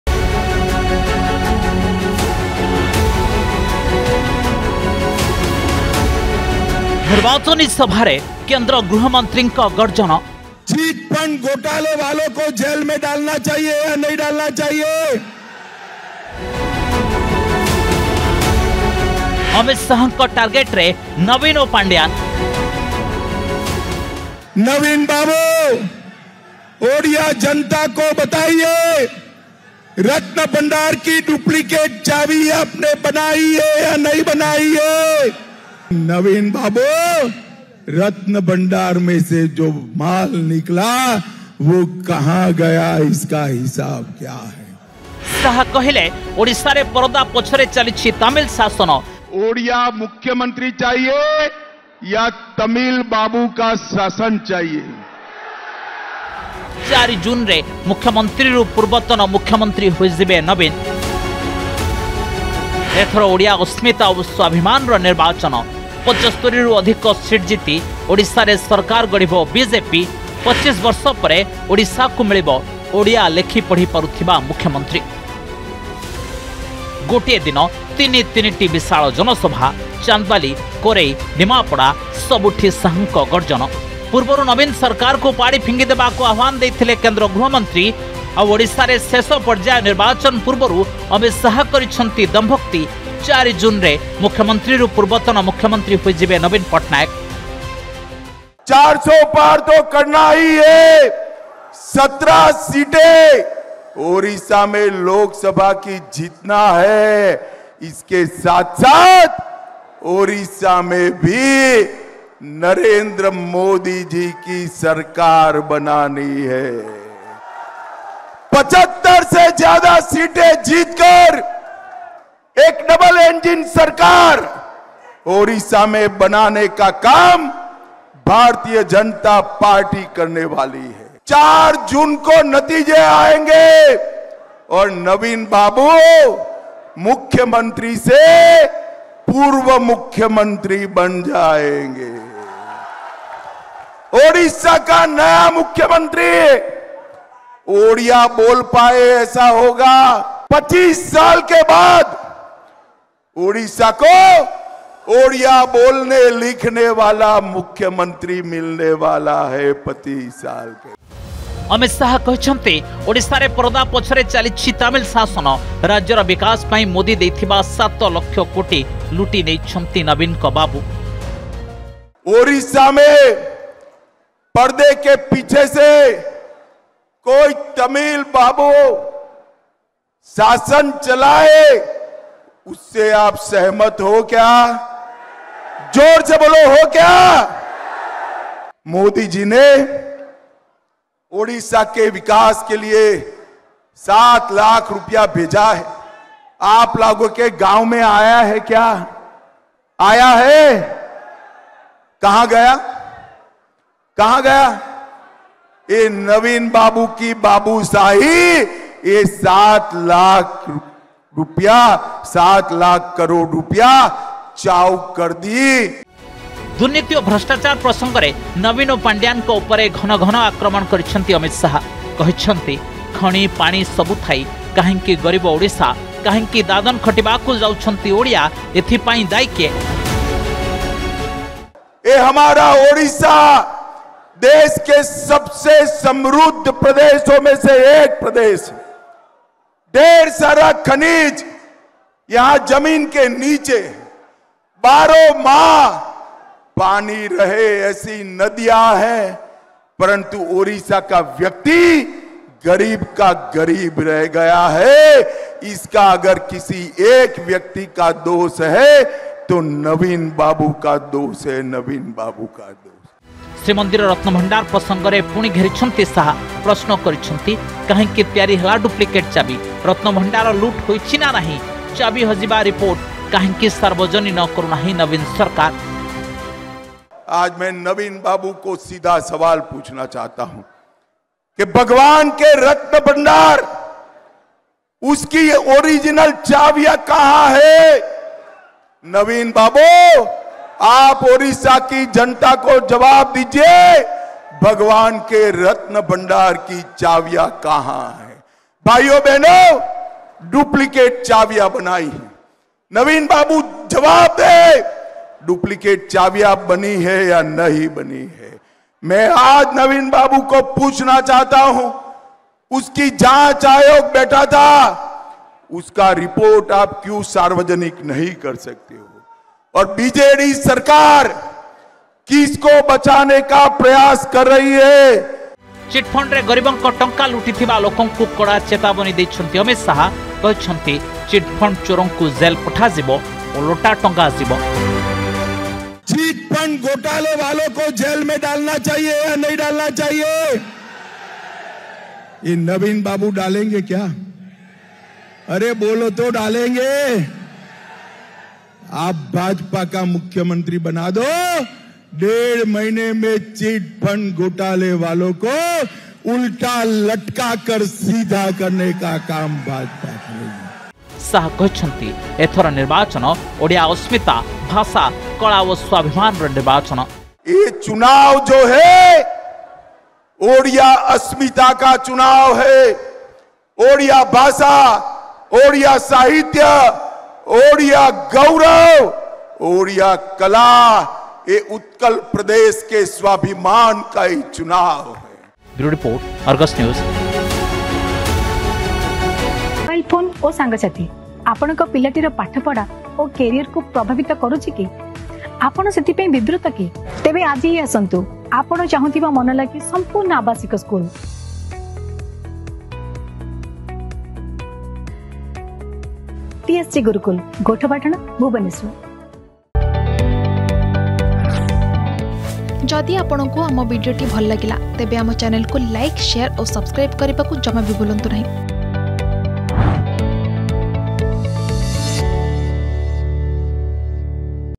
ନିର୍ବାଚନୀ ସଭାରେ କେନ୍ଦ୍ର ଗୃହମନ୍ତ୍ରୀଙ୍କ ଗର୍ଜନ || Amit Shah
ନିର୍ବାଚନୀ ସଭାରେ କେନ୍ଦ୍ର ଗୃହମନ୍ତ୍ରୀଙ୍କ ଗର୍ଜନ । ଅମିତ୍ ଶାହଙ୍କ ଟାର୍ଗେଟରେ ନବୀନ ଓ ପାଣ୍ଡିଆନ । ଓଡ଼ିଶାରେ ପରଦା ପଛରେ ଚାଲିଛି ତାମିଲ୍ ଶାସନ । ୪ ଜୁନରେ ମୁଖ୍ୟମନ୍ତ୍ରୀରୁ ପୂର୍ବତନ ମୁଖ୍ୟମନ୍ତ୍ରୀ ହୋଇଯିବେ ନବୀନ ।